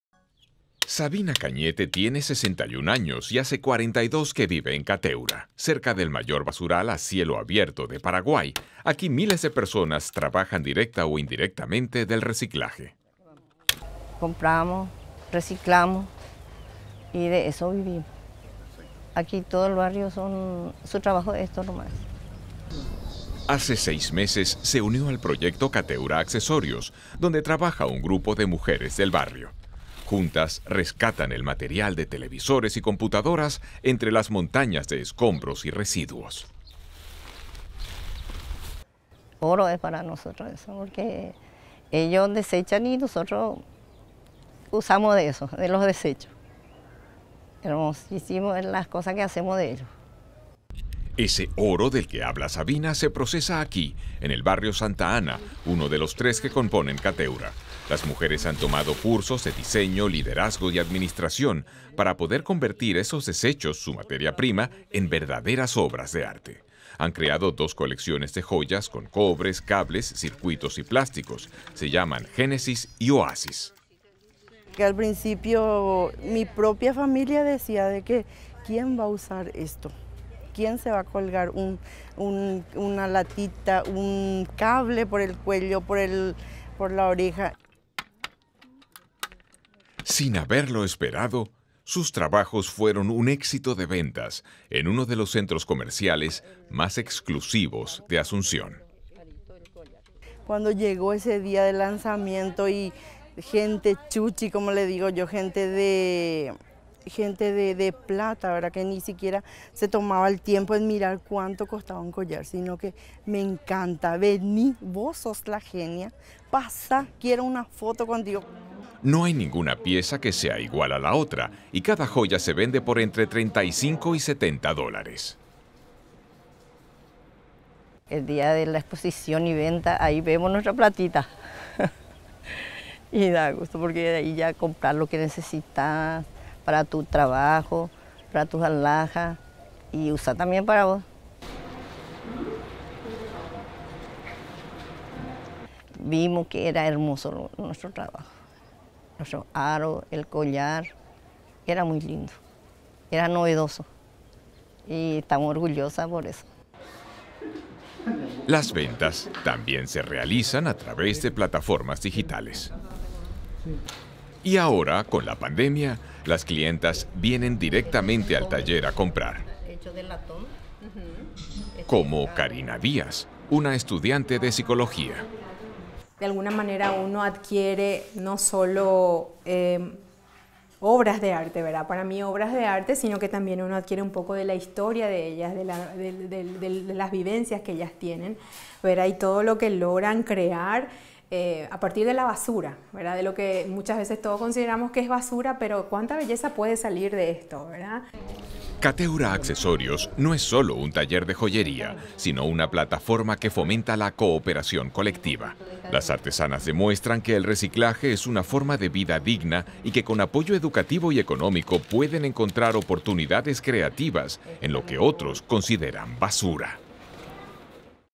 Construyendo joyas de la basura - Paraguay
Cateura es una pequeña ciudad de Paraguay, construida en torno al mayor vertedero de basura de la capital, Asunción. Las toneladas de desechos son también la forma de vida de miles de personas que se dedican a rescatar los objetos que puedan ser vendidos o reciclados. Allí, un grupo de mujeres demuestra que todo lo que tiramos puede tener una segunda vida, incluso mejor que la de antes.